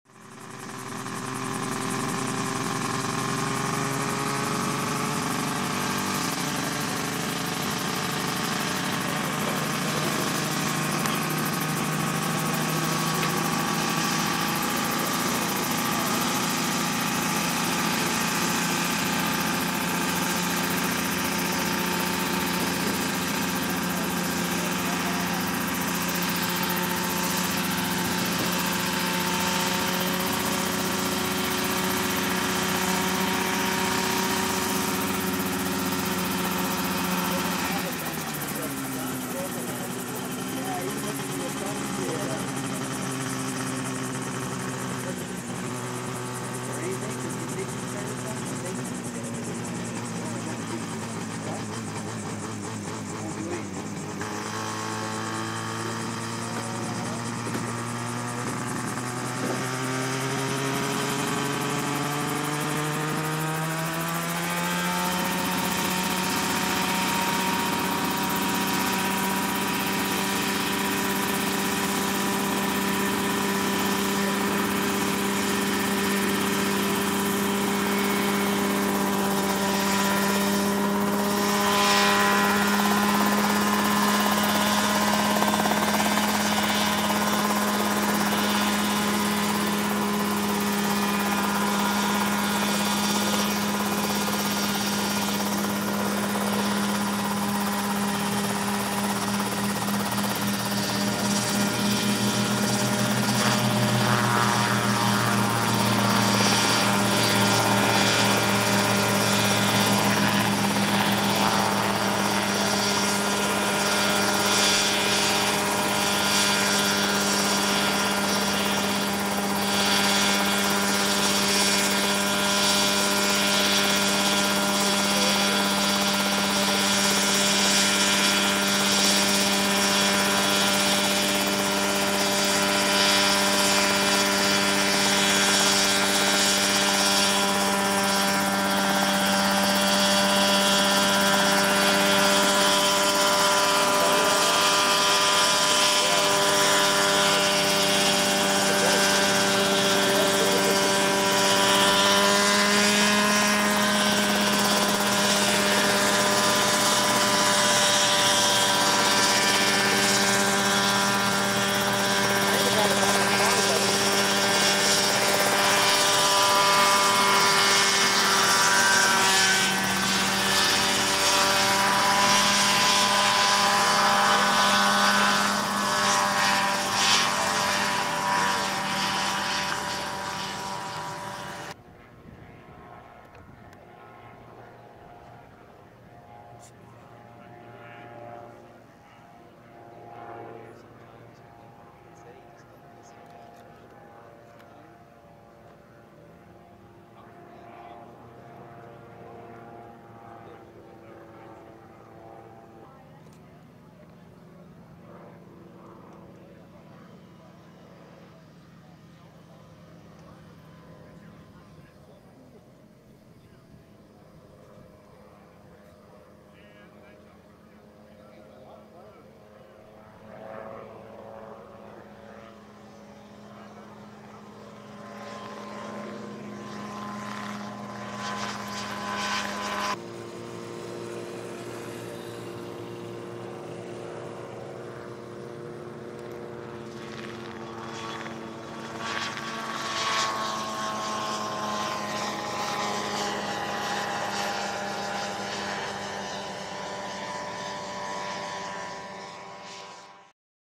Mosquito Helicopter
Here's some footage of a Mosquito helicopter I shot last year at a flyin.